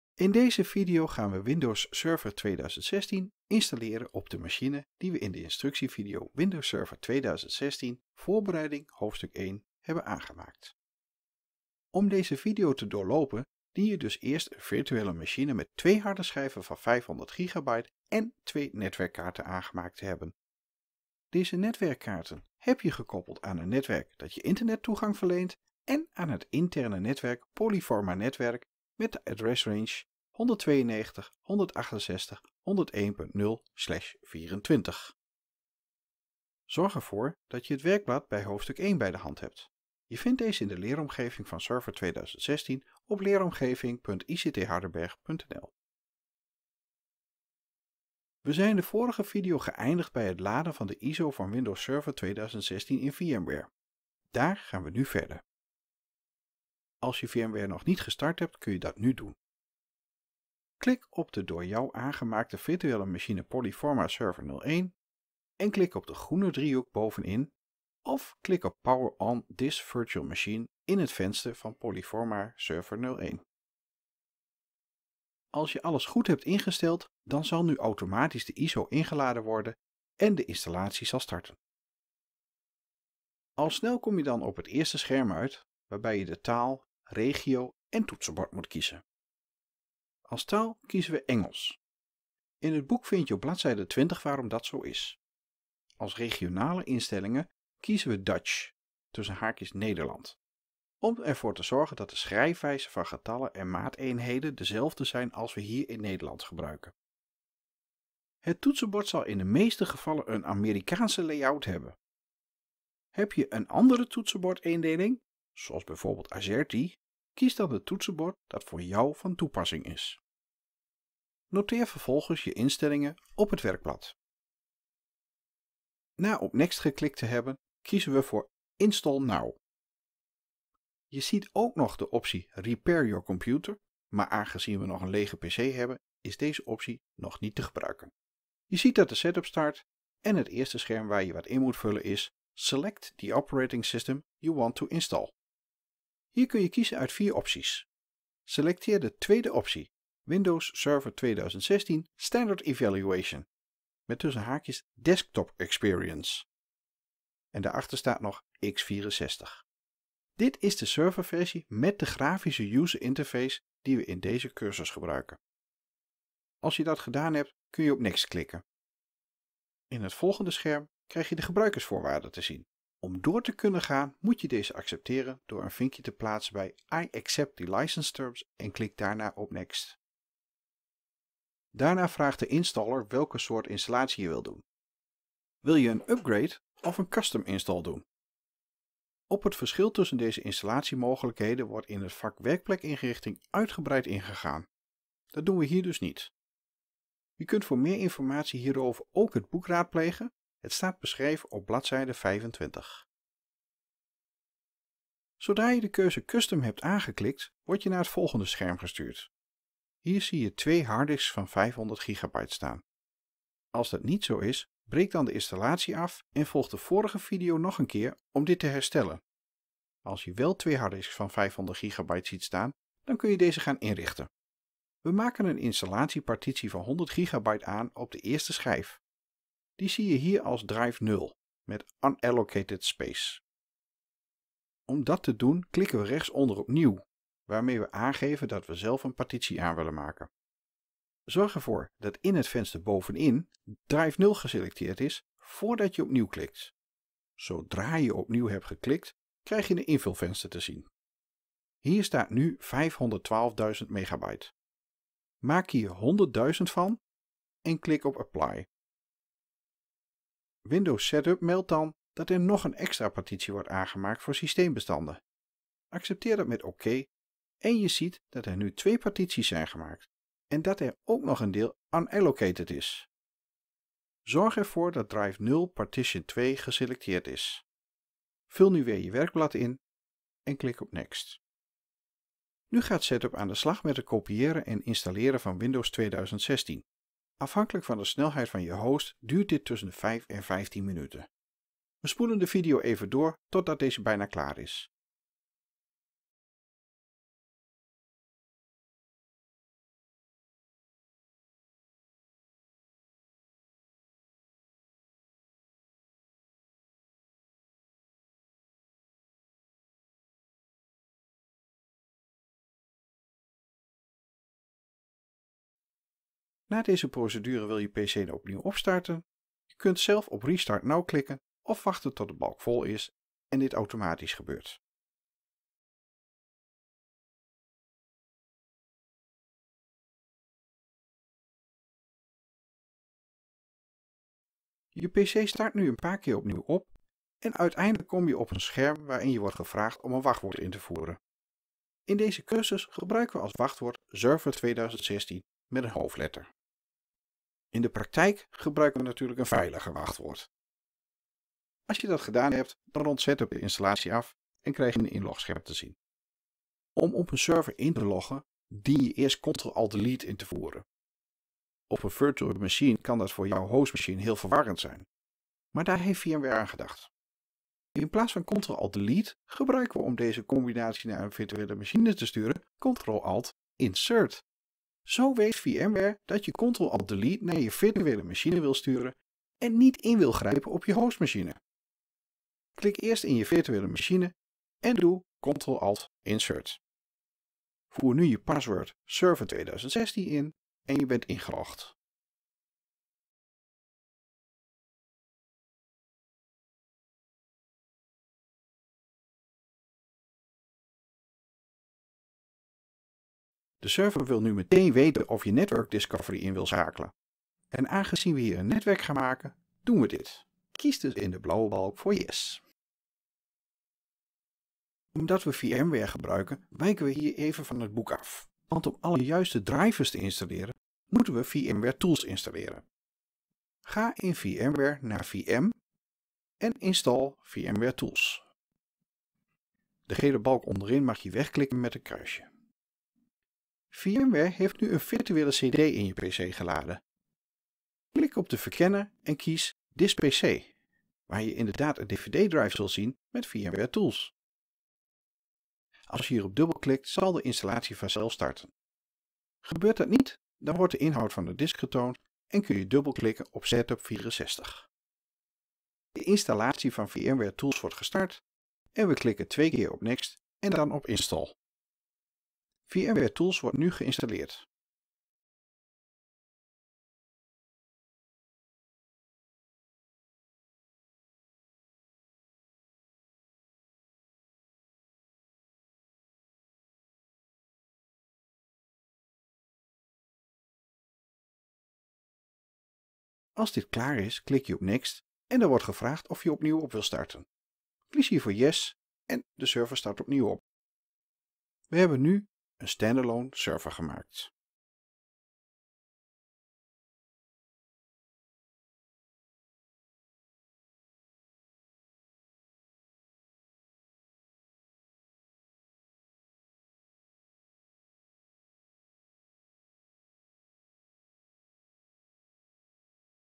2 Windows Server 2016 Installatie Server PFSV01
Installatie PoliForma Server 01 uit het boek netwerken met Server 2016 van Jan Smets in VMWare 15.5.

Voor gebruik op het Alfa College. Video gemaakt door Edwin Westerhoff.